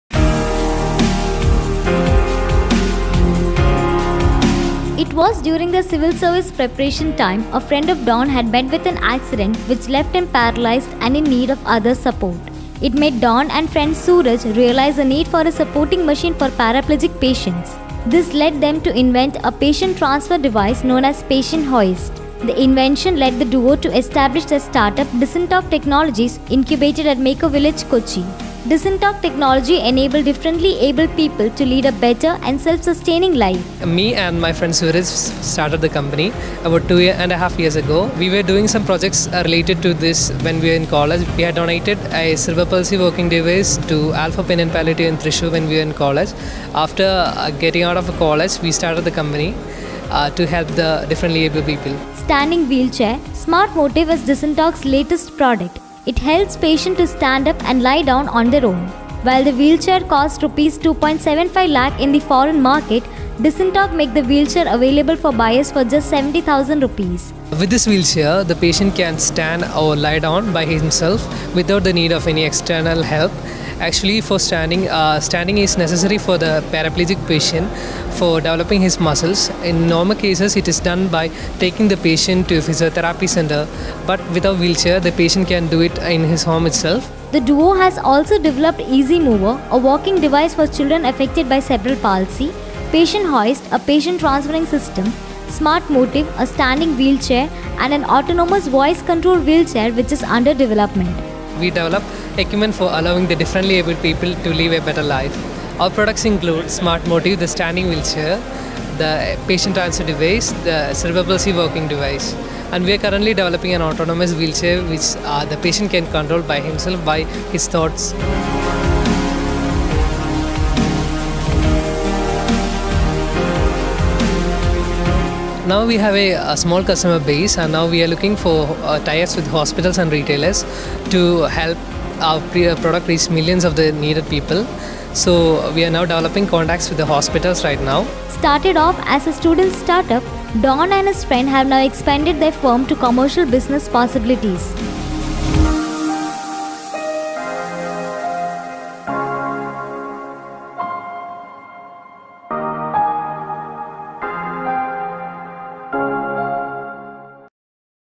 Desintox enables differently-abled people to lead a self-sustaining life | channeliam.com
Desintox Technologies Pvt Ltd, a startup founded by Don Paul and Sooraj Chandran, deals with the design, manufacture, and marketing of assistive equipment to help the differently abled people. Kerala-based Desintox Technologies is incubated at maker village Kochi.

While pursuing BTech at the Government Engineering College in Thrissur, Don Paul and Rahul developed a walking device for children affected by cerebral palsy as part of their college project. After their graduation, one of their friends met with an accident and the duo realized the need of assistive equipment for paraplegic patients and thus developed a patient transfer device, Patient Hoist. Currently the firm has 5 members and so far they have developed a walking device for children affected by cerebral palsy, patient transferring system, Patient Hoist, standing wheel chair Smart Motive and an autonomous voice controlled wheelchair which is under development.

Channeliam.com - A leading digital video media firm for startups and entrepreneurs